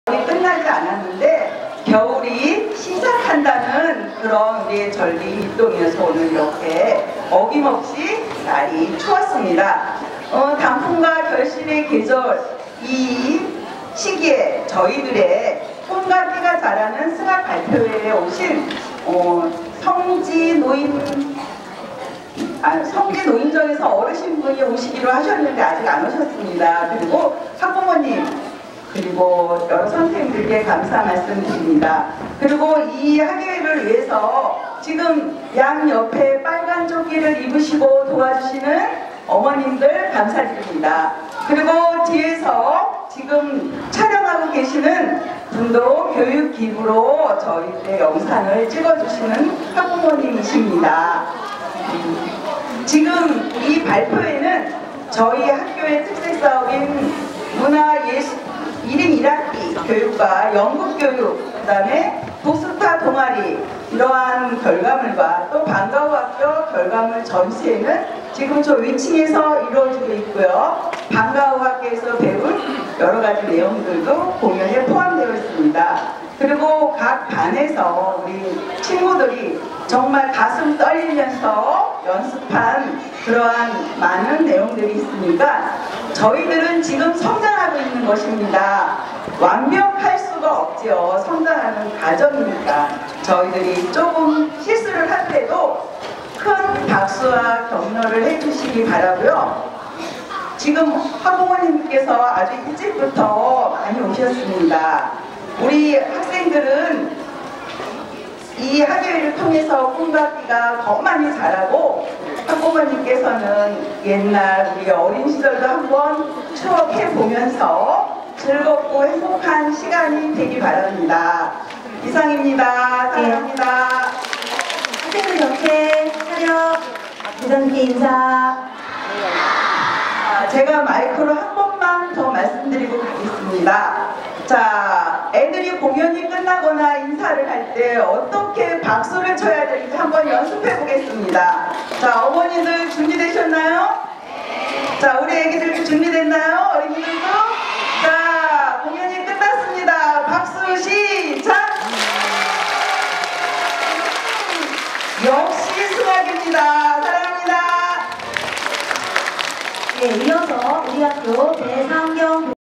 2019 인천 승학초등학교 학예발표회 00 교장선생님 인사 교감선생님 학교 발표
2019 인천 승학초등학교 학예발표회 00 교장선생님 인사 교감선생님 학교 발표

못오신 부모님을 위해, 그리고 참석하셨어도 다시 추억을 열어볼 때를 위해...

좋아요!  + '구독' 버튼 눌러주시고 + '댓글' 달아주세요. 승학초등학교 영상은 계속 올려드립니다.